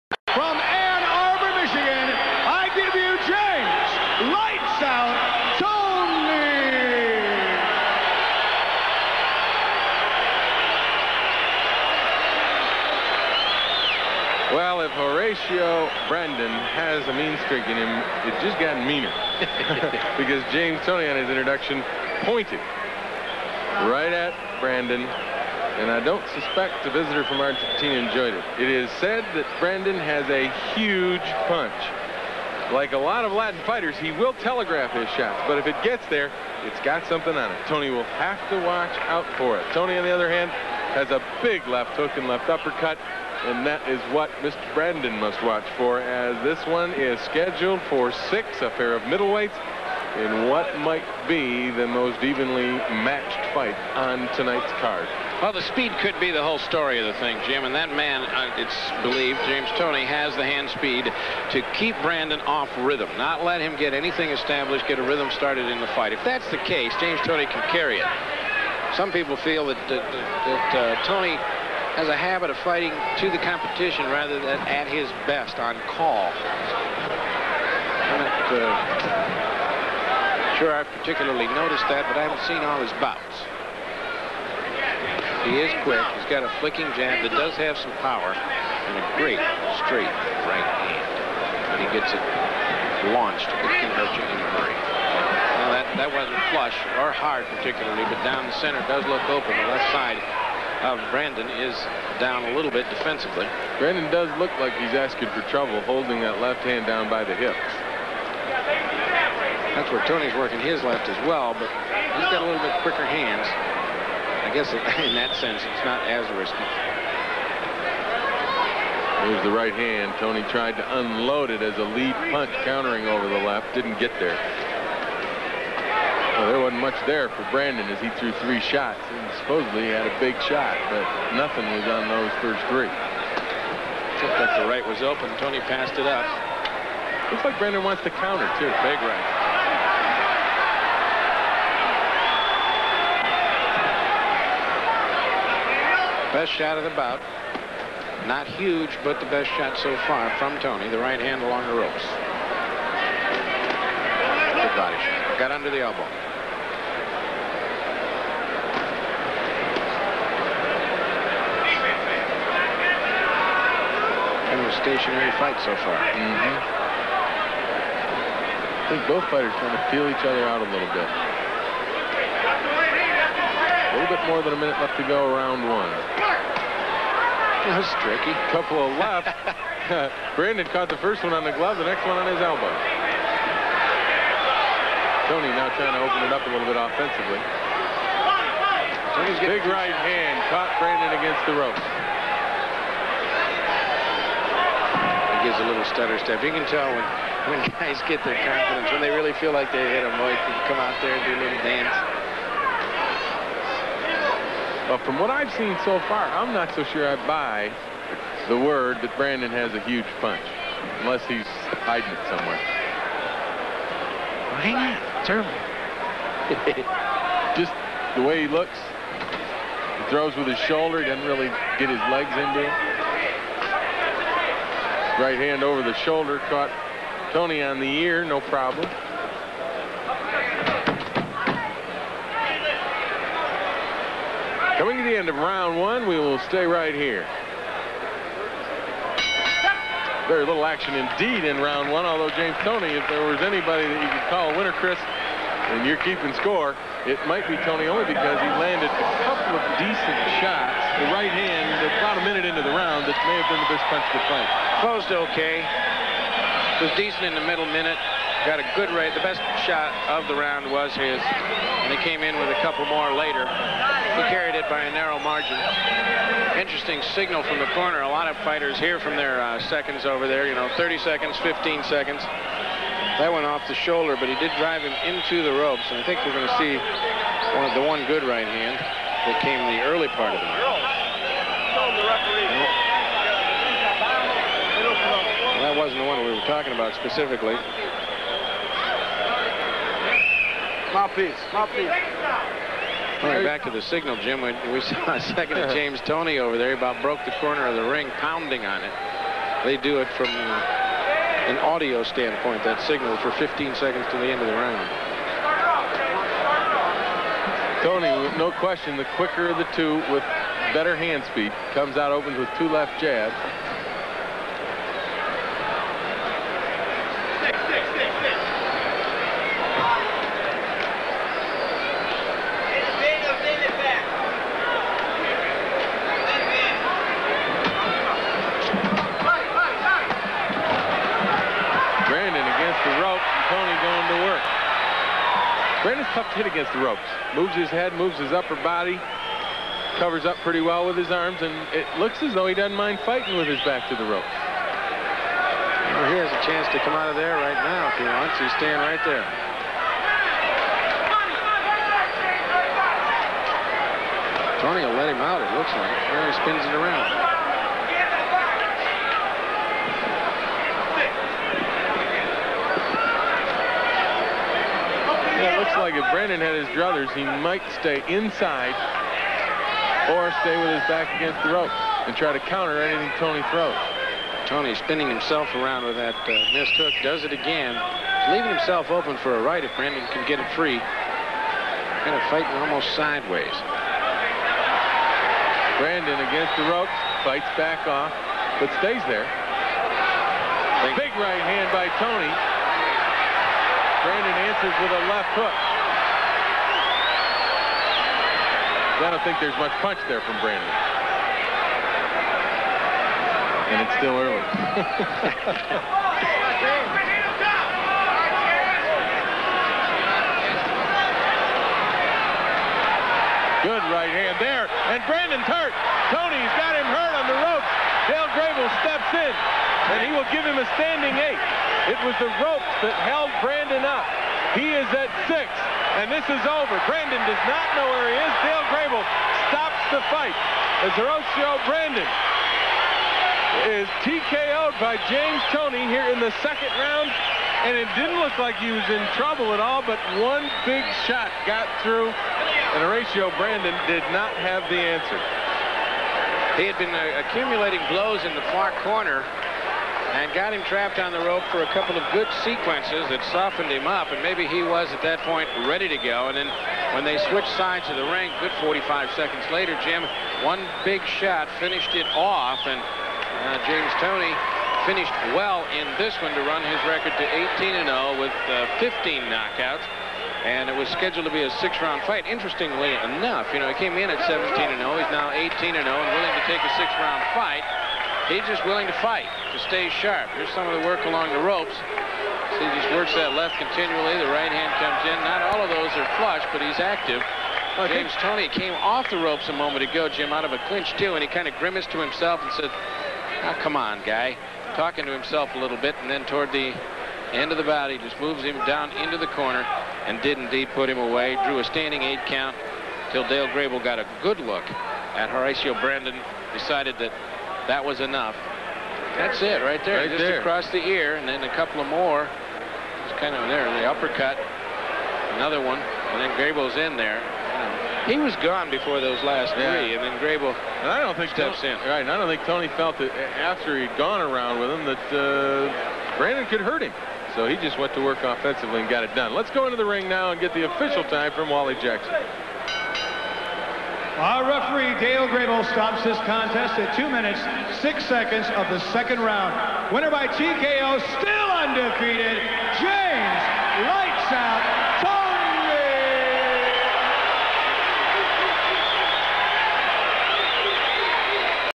James Toney vs Horacio Brandan (full fight)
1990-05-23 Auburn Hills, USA
10 rounds middleweight fight